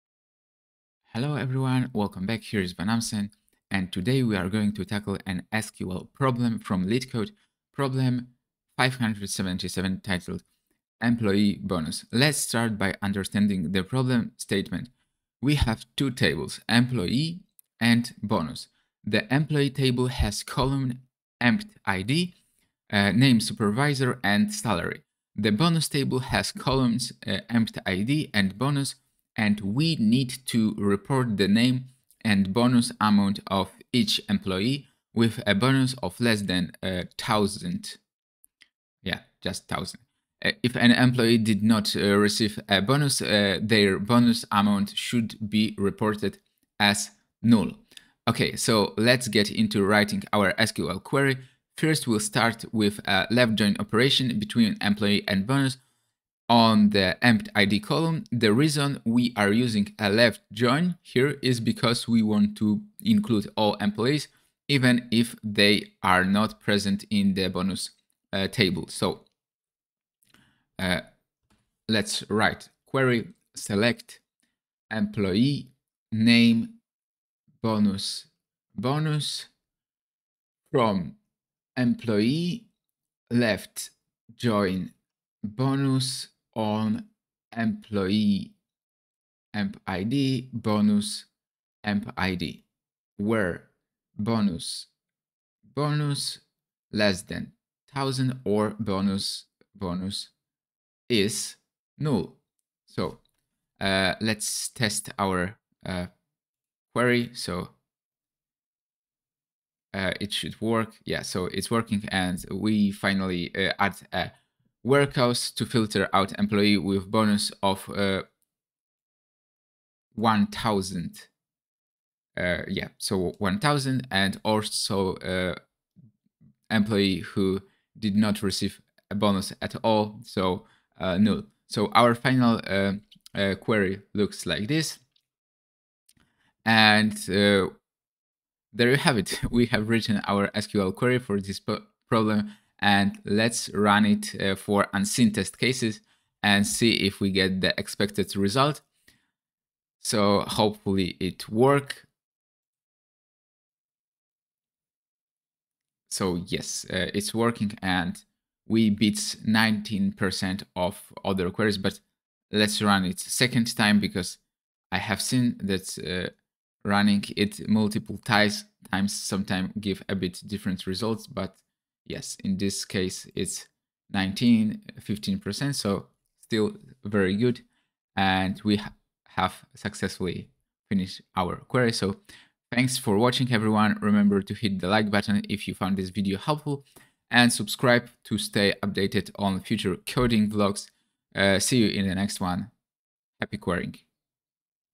Solving LeetCode 577: Employee Bonus with SQL | vanAmsen Coding Vlog
Welcome back to another coding vlog with vanAmsen! Today, we're diving into SQL with LeetCode problem 577: Employee Bonus. This problem is a great way to understand how to work with JOIN operations and WHERE clauses in SQL.

In this video, I'll guide you step-by-step through the process of understanding the problem, writing the SQL query, and testing our solution. We will be using a LEFT JOIN operation to connect the Employee and Bonus tables, and a WHERE clause to filter out employees with a bonus of 1000 or more.

I'd love to hear about your SQL journey and answer any questions you might have.

0:00 - Introduction
1:05 - Implementation
1:46 - Testing